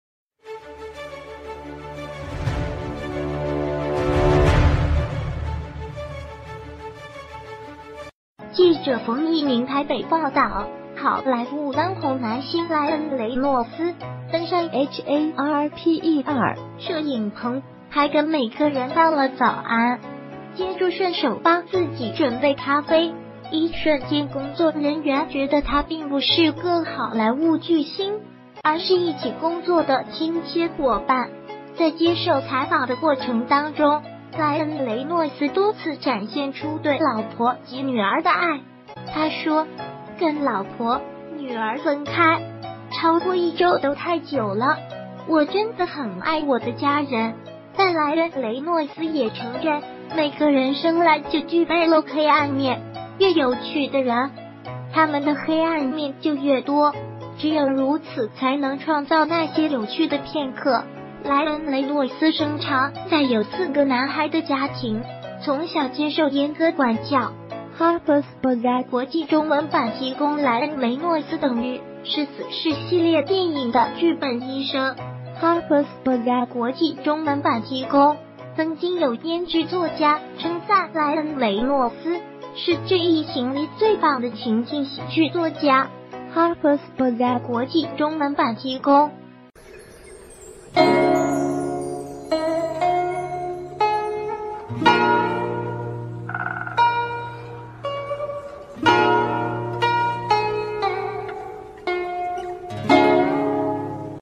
萊恩雷諾斯私底下原來心存諧星魂
〔記者馮亦寧／台北報導〕好萊塢當紅男星萊恩雷諾斯登上《Harper&quo;s BAZAAR》三月號封面，這是整個工作團隊越洋拍攝的成果，但沒想到老婆卻在現場爆料出他私底下的面目。

這次《Harper's BAZAAR》封面是台灣工作團隊的努力，萊恩雷諾斯其實是個有很多不一樣面貌的人，老婆就偷偷告訴工作人員：「他很喜歡自己寫一些段子，總是特地拿來逗我笑，這就是我最愛他的地方！」

其實萊恩雷諾斯的童年家庭生活並不圓滿，「幽默感」就像是他自我保護的一面，而現在樂當女兒奴的他，竟然可以半夜起床五次為孩子換尿布，連太太都忍不住佩服他。

萊恩雷諾斯的魅力就像太陽依樣，受到光亮與黑暗的交錯，讓他在風趣幽默之外，多了一層神秘難解的魅力，反倒讓人更想細細探究。

拍攝當天，他穿著白色T恤與淡藍色毛衣來到攝影棚，還跟每個人道了早安，接著順手幫自己準備咖啡，一瞬間工作人員覺得他並不是個「好萊塢巨星」，而是一起工作的親切夥伴。

在接受採訪的過程當中，萊恩雷諾斯多次展現出對老婆及女兒的愛，他說：「跟老婆、女兒分開，超過一週都太久了，我真的很愛我的家人。」但萊恩雷諾斯也承認：「每個人生來就具備了黑暗面，越有趣的人，他們的黑暗面就越多，只有如此才能創造那些有趣的片刻」。萊恩雷諾斯生長在有四個男孩的家庭，從小接受嚴格管教。（《Harper’s BAZAAR》國際中文版提供）

萊恩雷諾斯等於是《死侍》系列電影的劇本醫生。（《Harper’s BAZAAR》國際中文版提供）

曾經有編劇作家稱讚萊恩雷諾斯是這一行裡最棒的情境喜劇作家。（《Harper’s BAZAAR》國際中文版提供）